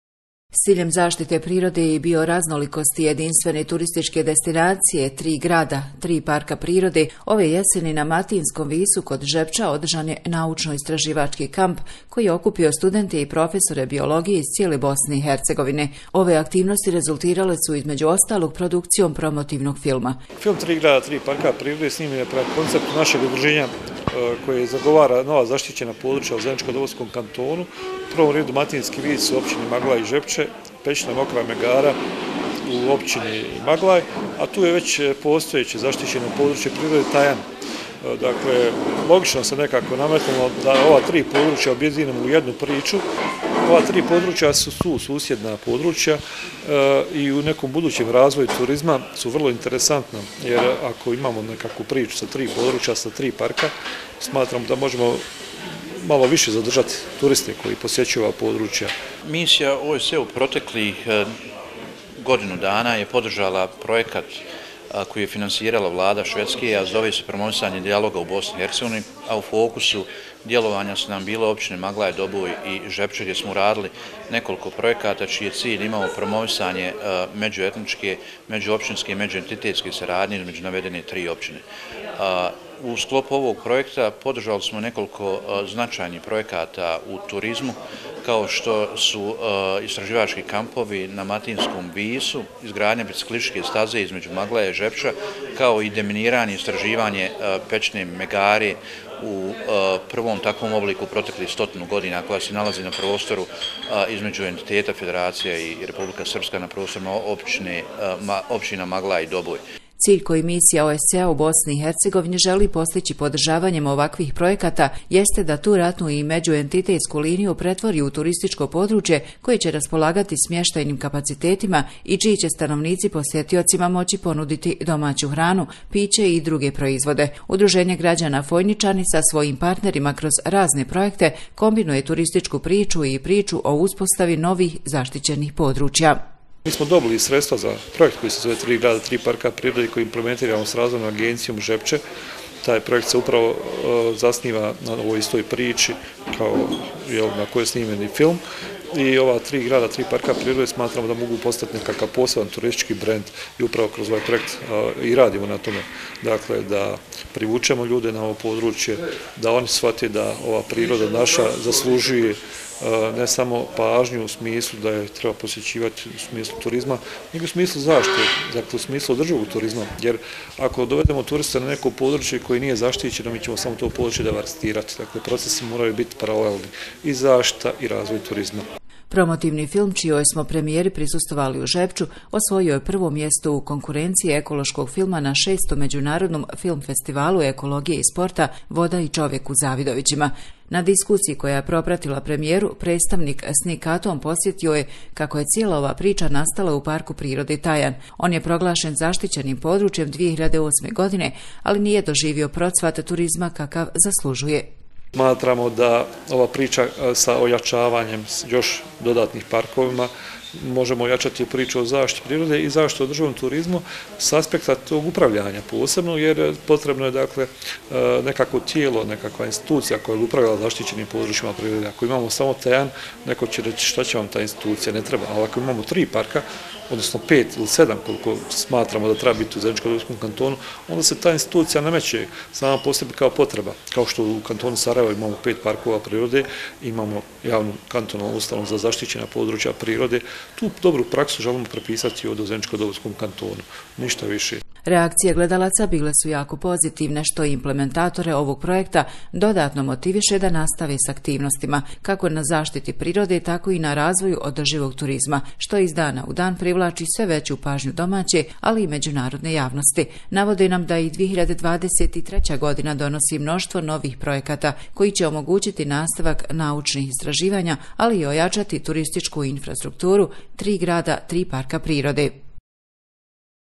Hronika: Zavidovići 300 - Premijera "Tri grada, tri parka prirode"
Promotivni video „3 grada, 3 parka prirode“, snimljen uz podršku OSCE Misije u Bosni i Hercegovini i Švedske, prema ideji i konceptu Udruženja Fojničani Maglaj, imao je svoje premijere u Zavidovićima, Žepču i Maglaju. Sam projekat kroz koji je snimljen film, fokusiran je na uspostavu i razvoj  jedinstvene turističke destinacije na području navedene tri općine.